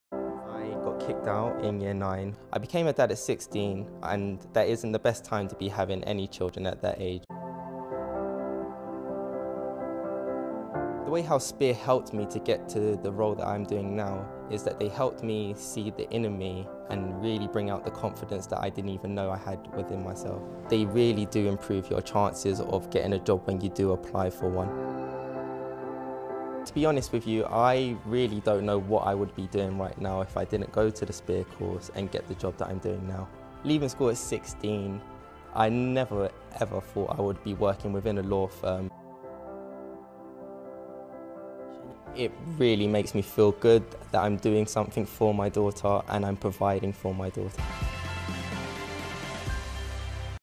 Damaris' story
Damaris was kicked out of school and became a father at 16. He explains how the Spear Programme brought out confidence he didn't know he had.